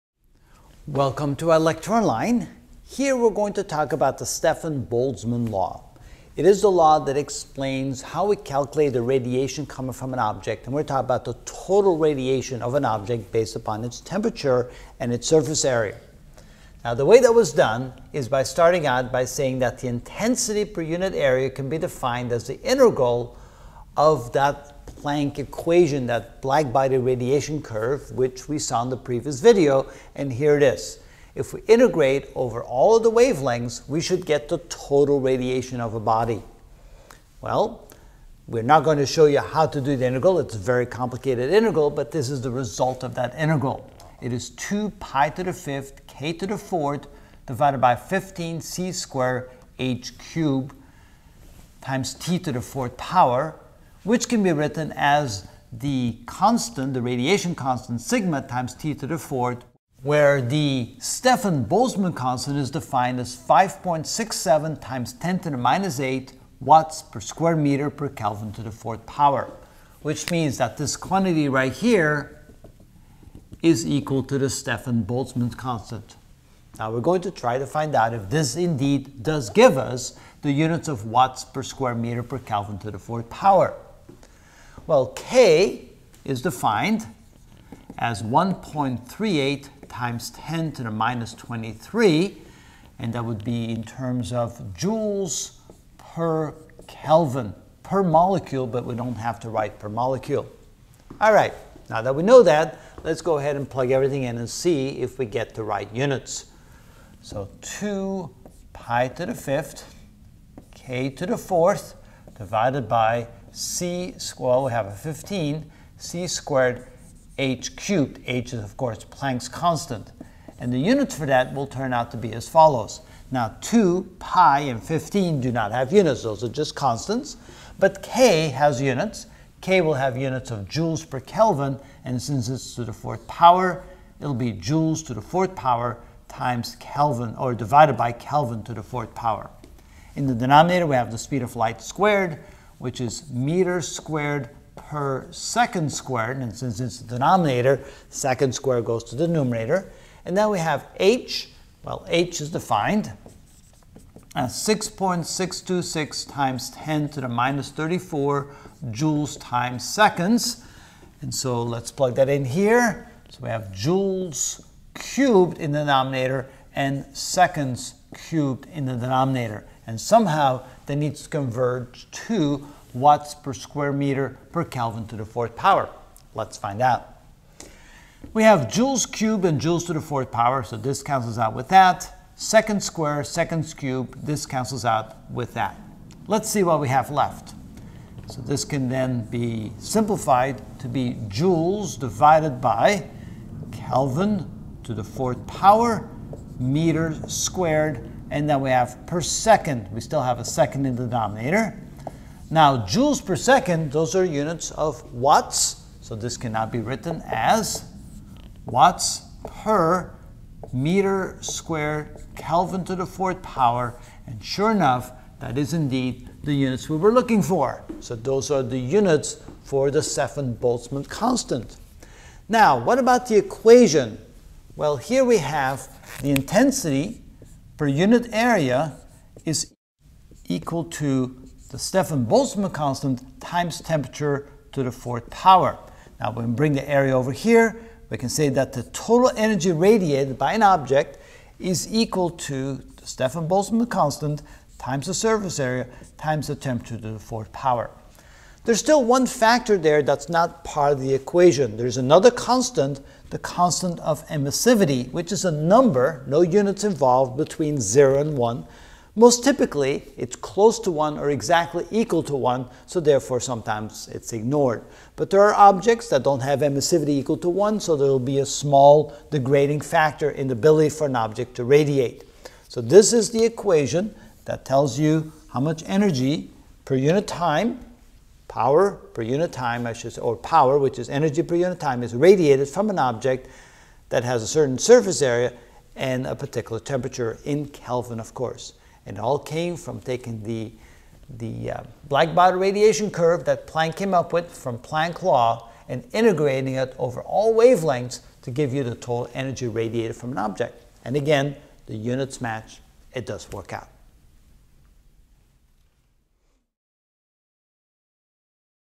Physics CH 0.5: Standard Units (38 of 41) Stefan Boltzmann Law
In this video I will explain and derive the units of the Stefan Botzmann Law of the total radiation coming from an object base on its temperature and its surface area.